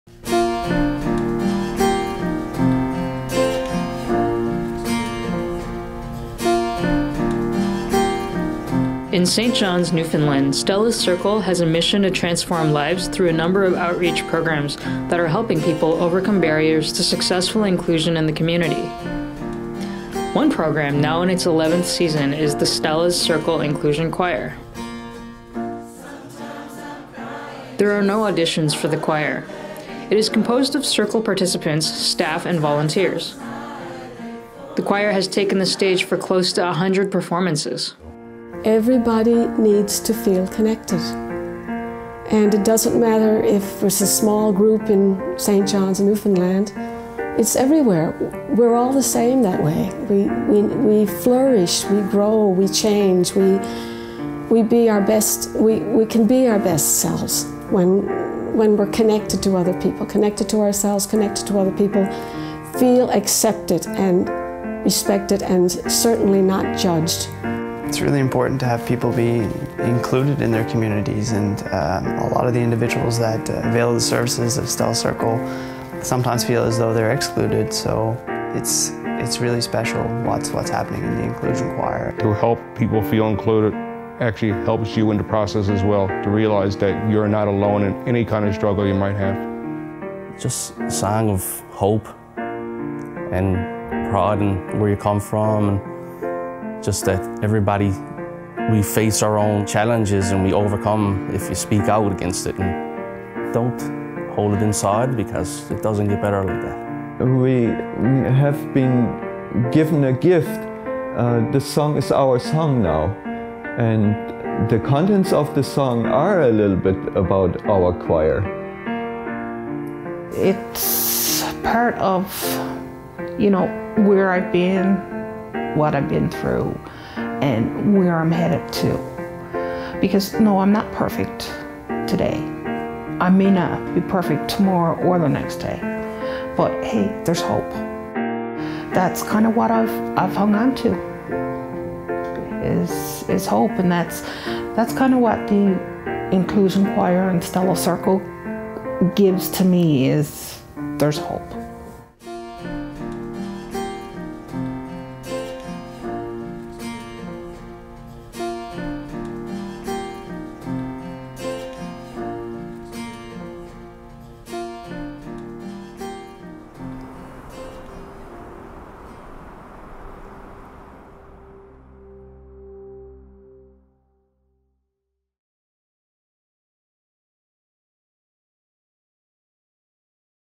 Minutes for Mission 2019 - Singing for Inclusion
Minutes for Mission: September 8, 2019. Stella’s Circle has a mission to transform lives through a number of outreach programs that are helping people overcome barriers to successful inclusion in the community. The Inclusion Choir, now in its 11th season, connects all of its participants through the power of music. Supported through your generous donations to the Mission & Service of The United Church of Canada.